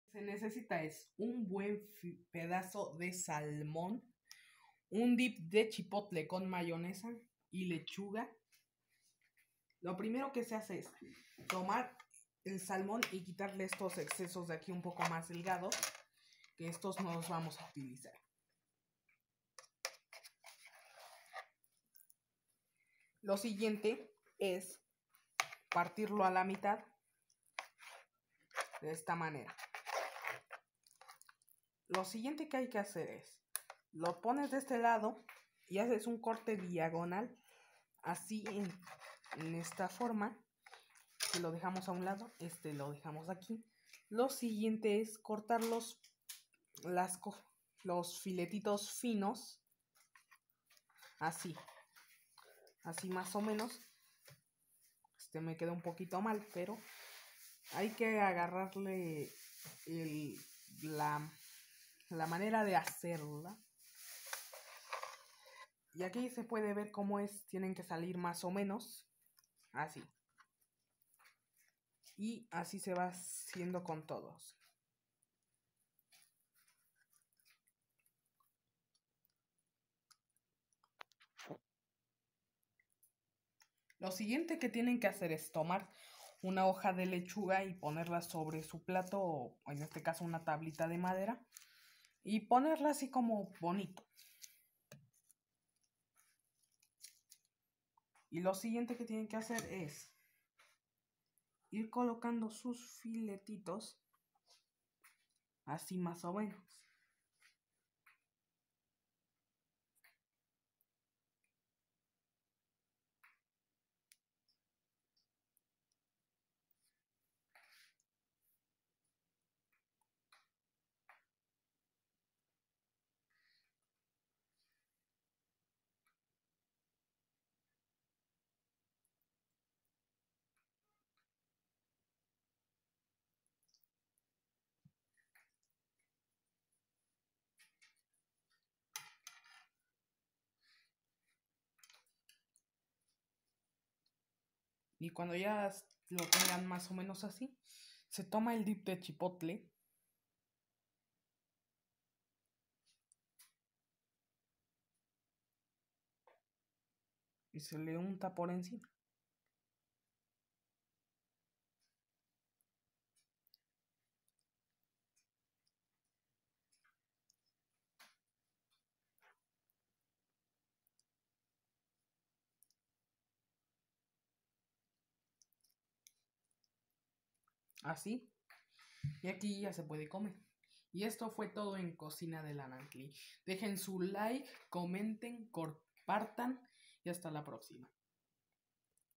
!EXQUISITO! PLATILLO JAPONES CON SALMON CRUDO.
INGREDIENTES:
1/4 DE SALMON SIN PIEL.
1 LECHUGA ITALIANA.
DIP DE CHIPOTLE
DELICIOSO